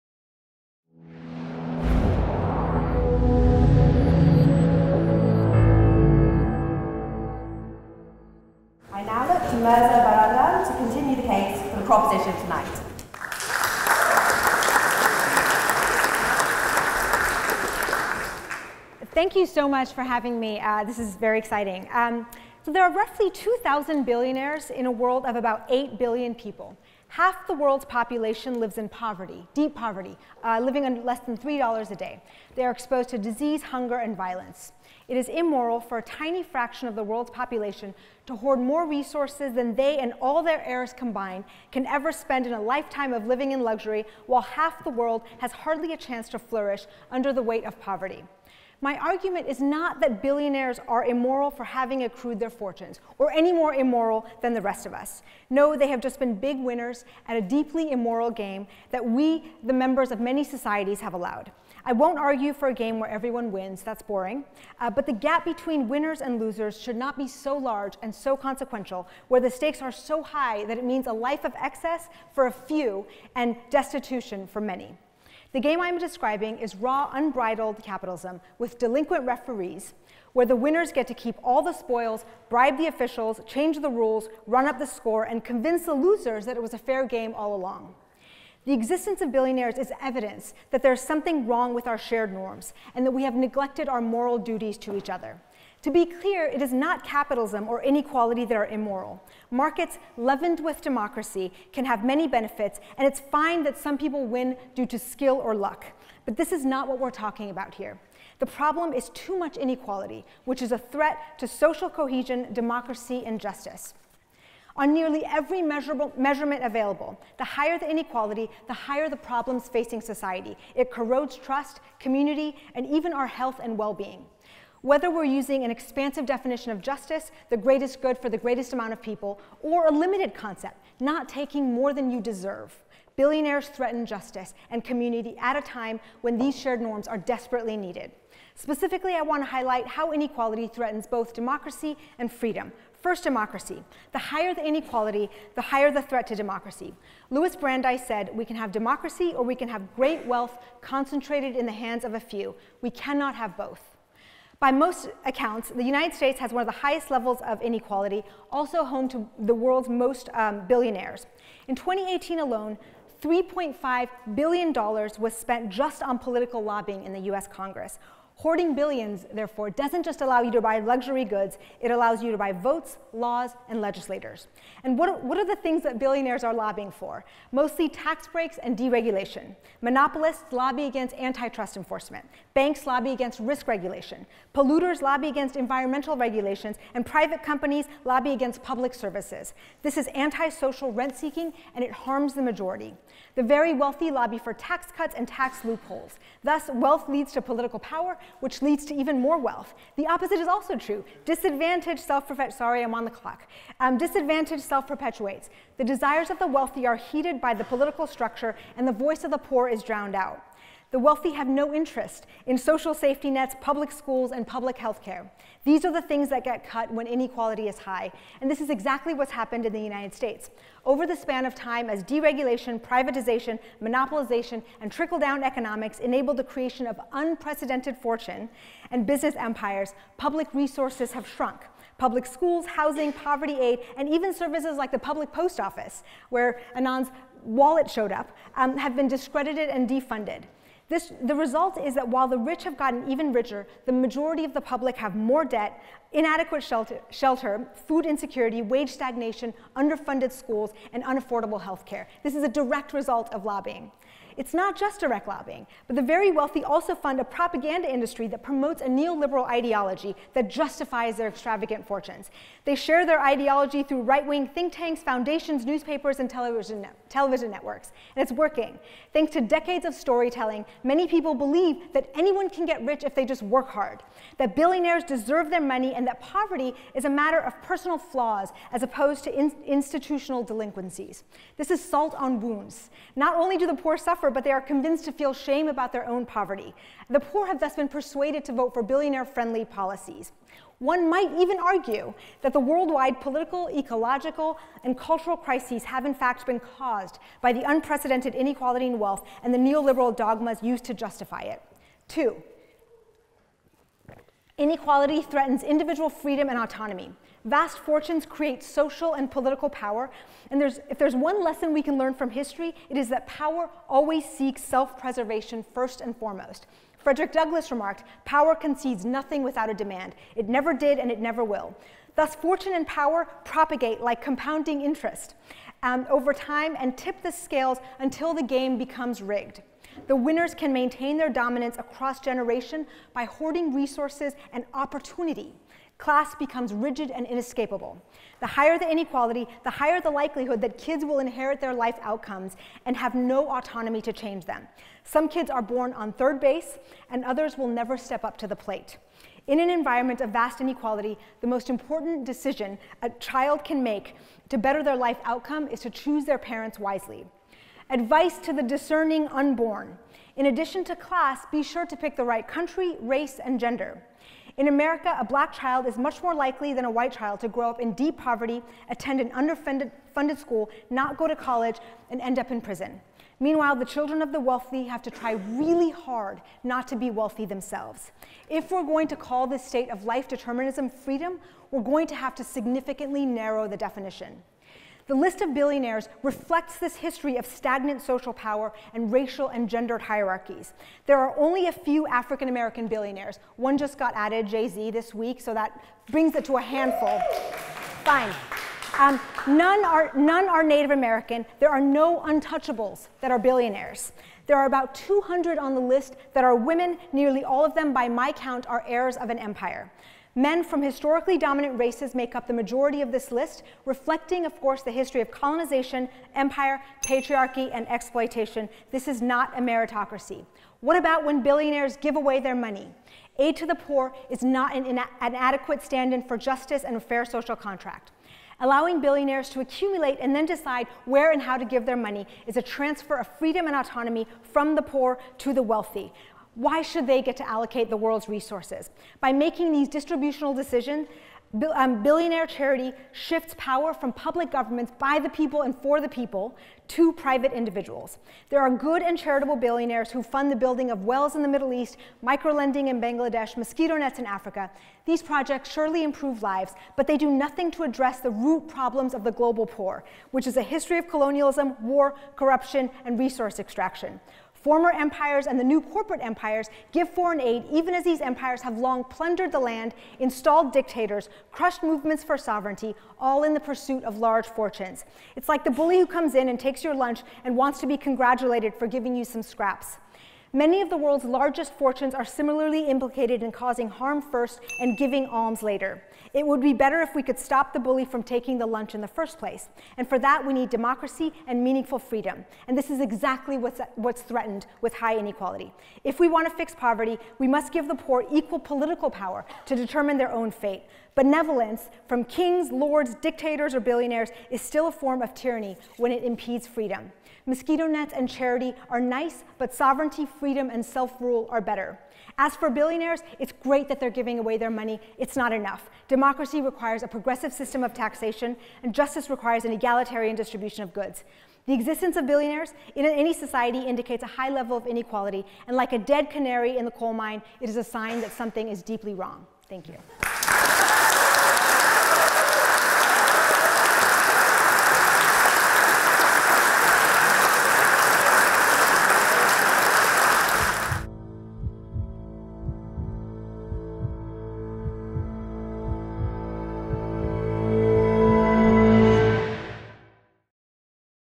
Prof Mehrsa Baradaran | It Is Immoral To Be A Billionaire (7/8) | Oxford Union
The Motion: This House Believes It Is Immoral To Be A Billionaire.

Professor Mehrsa Baradaran closes the case for the Proposition, as the seventh speaker of eight in the debate.

Professor Mehrsa Baradaran is a law professor at the University of Georgia specialising in banking law, she has written two books, How the Other Half Banks and The Color of Money.